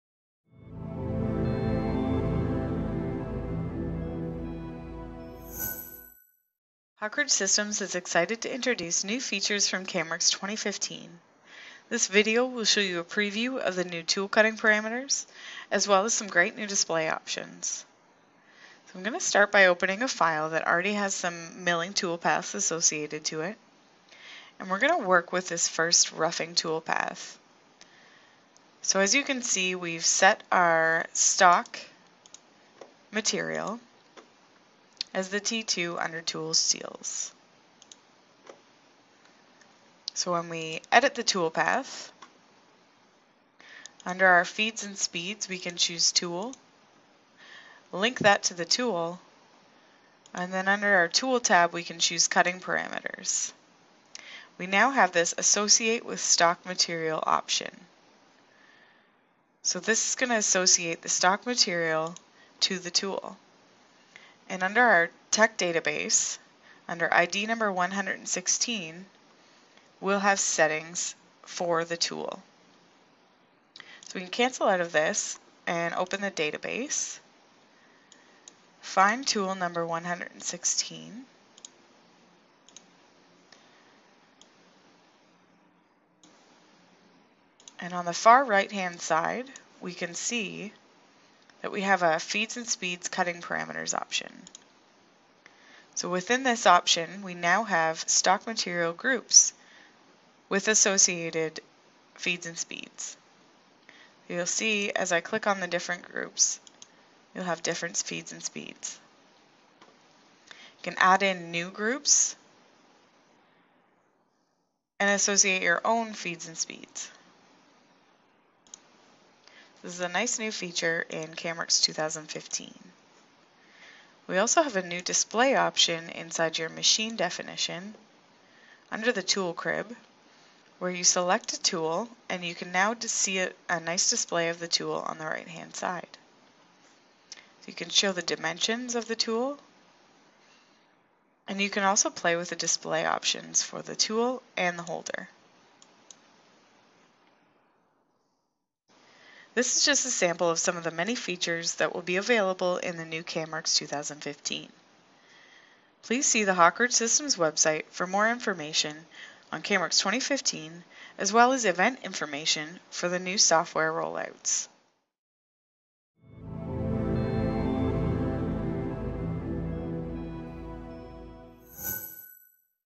CAMWorks - New in 2015: Multi Feeds and Speeds for Tools and Display Enhancements
This video gives you a preview of the new multi speeds and feeds functions in your mill tool settings. You will also see some new display enhancements for tooling and technology database data.